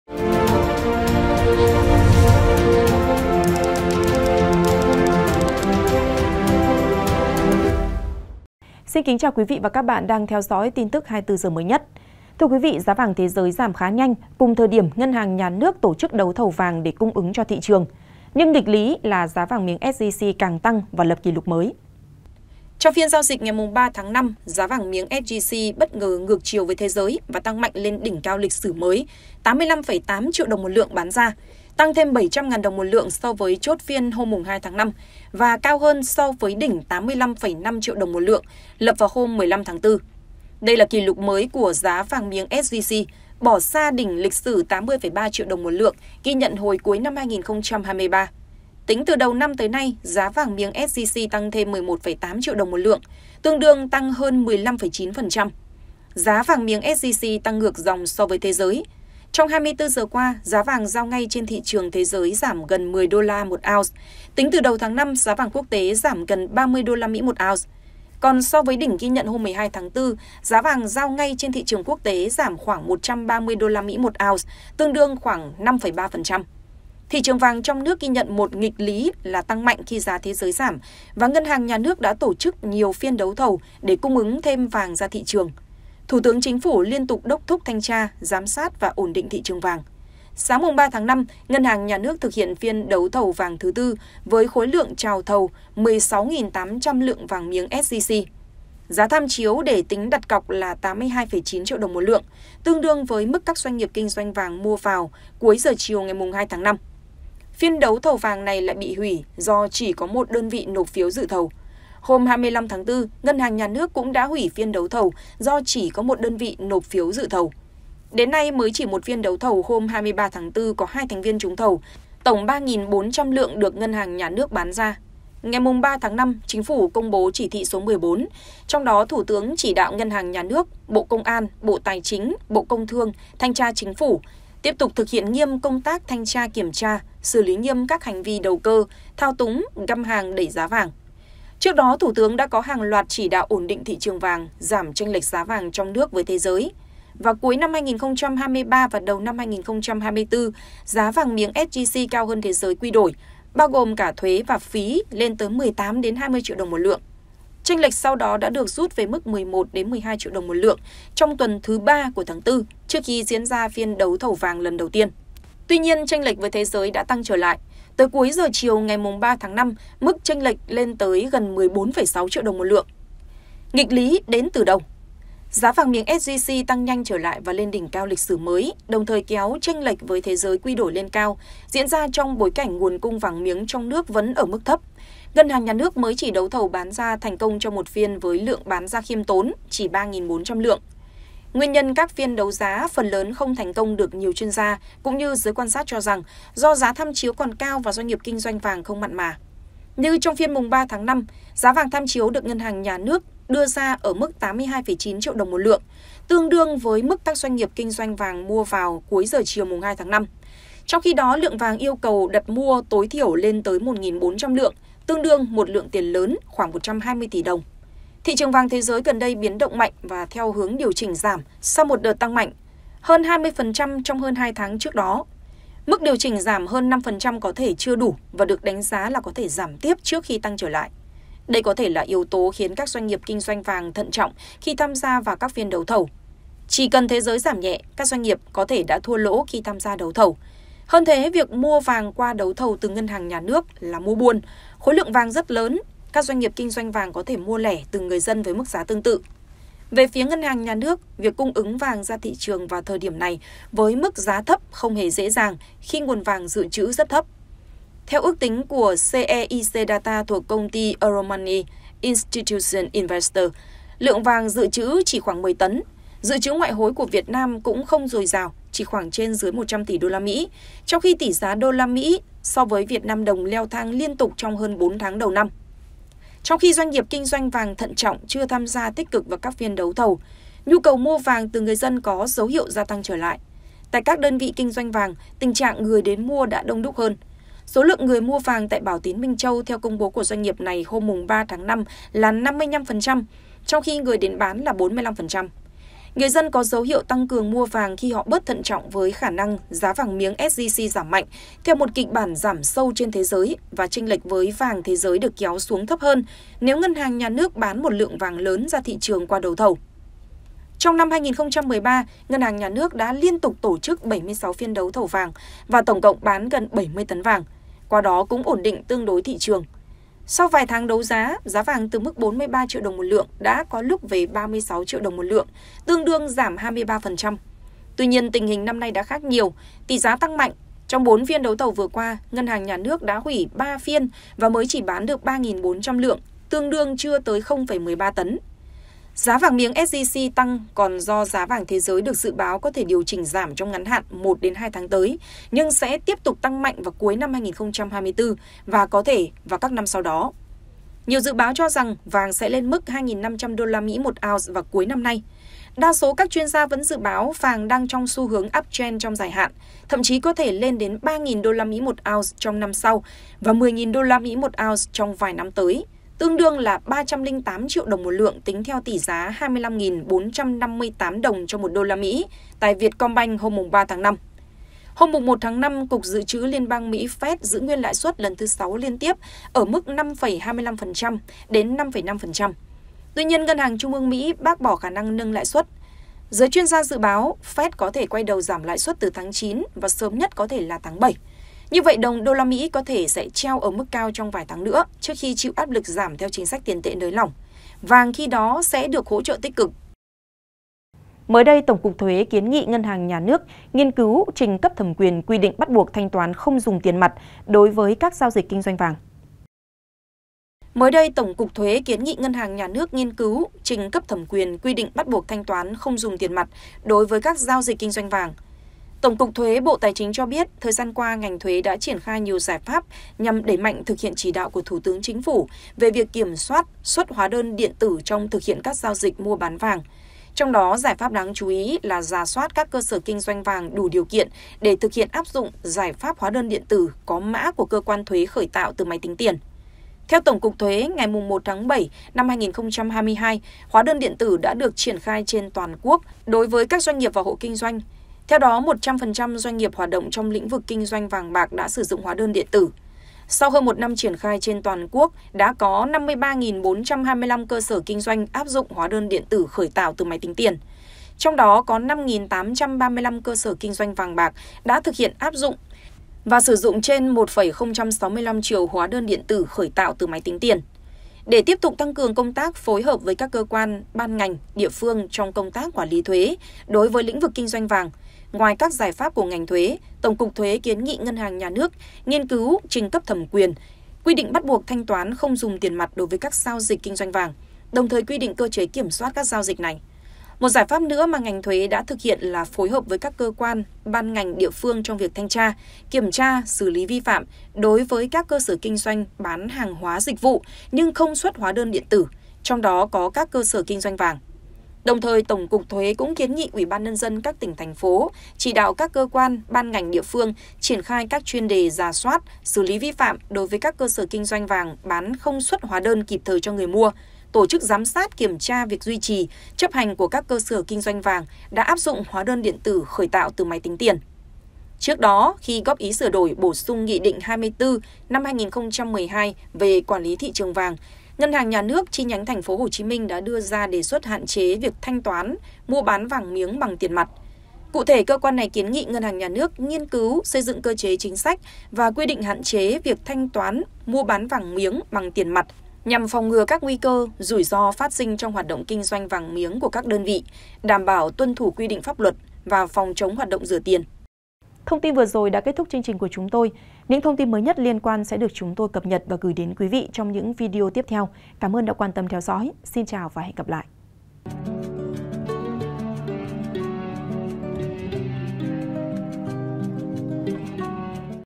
Nghịch Lý NHNN Đấu Thầu Vàng Tăng Cung Nhưng Giá Vàng SJC Vẫn Tăng Lập Kỷ Lục Mới | SKĐS
SKĐS | Giá vàng thế giới giảm khá nhanh, cùng thời điểm Ngân hàng Nhà nước tổ chức đấu thầu vàng để cung ứng cho thị trường, nhưng nghịch lý là giá vàng miếng SJC càng tăng và lập kỷ lục mới.